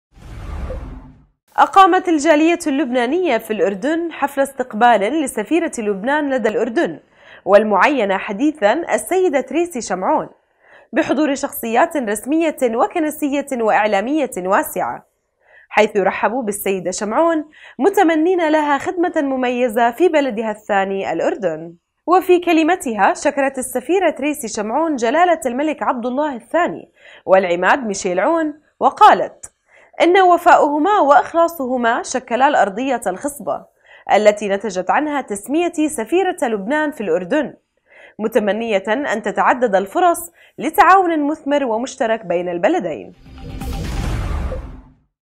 نورسات الأردن : أخبار - تعيينُ السيدة تريسي شمعون سفيرةَ لبنان لدى الأردن 2017
اقامت الجاليةُ اللبنانية في الأردن حفلَ استقبالٍ لسفيرةِ لبنان لدى الأردن والمعينة حديثاً السيدة تريسي شمعون .. بحضورِ شخصياتٍ رسميةٍ وكنسيةٍ واعلاميةٍ واسعة.. حيثُ رحبوا بالسيدة شمعون متمنينَ لها خدمةً مميزة في بلدِها الثاني الأردن
وفي كلمتِها شكرت السفيرة تريسي شمعون جلالةَ الملك عبدالله الثاني والعماد ميشال عون، وقالت إنَّ "وفاءهُما وإخلاصُهما شكلا الأرضيةَ الخصْبة، التي نتجت عنها تسميتي سفيرةً للبنان في الأردن، متمنيةً أن تتعددَ الفرص لتعاونٍ مثمرٍ ومشتركٍ بين البلدين".